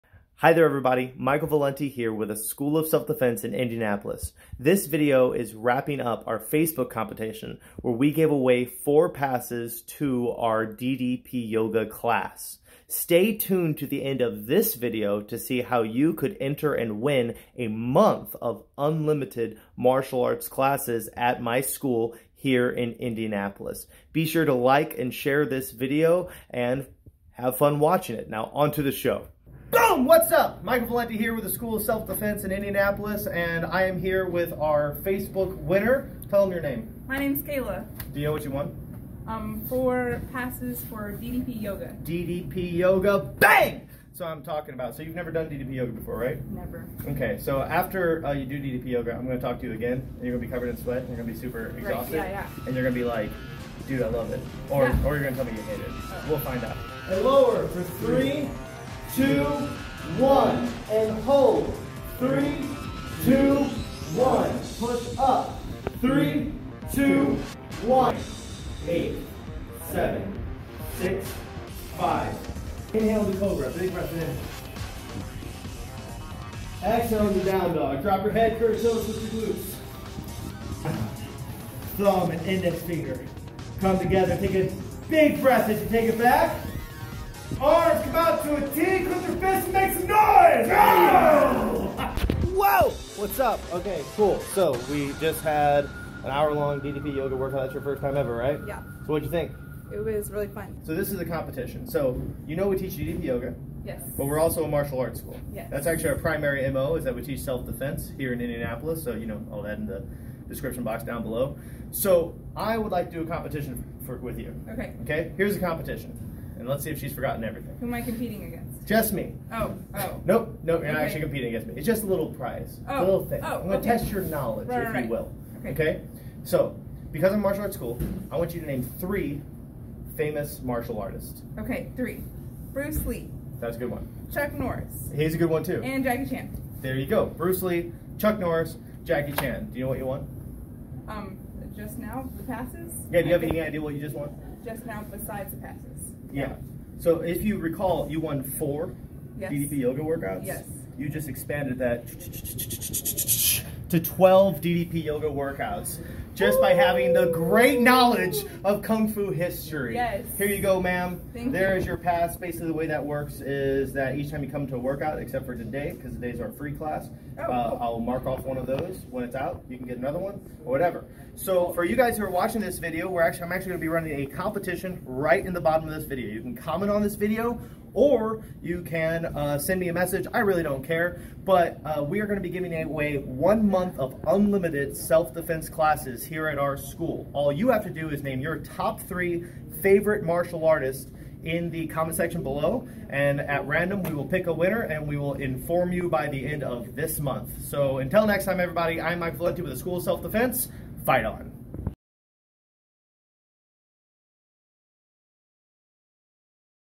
Win 1 Month of Martial Arts
Comment to Win
Prize: 1 month of unlimited martial arts classes at The School of Self Defense
How to Enter: List your top 3 favorite martial artist in the comment section.
Contest ends: 01/31/2020
Winners will be contacted on January 31st  and featured on our Facebook Page. If we do not hear back from you in 24 hours we will move on and pick a new winner. Good Luck to everyone.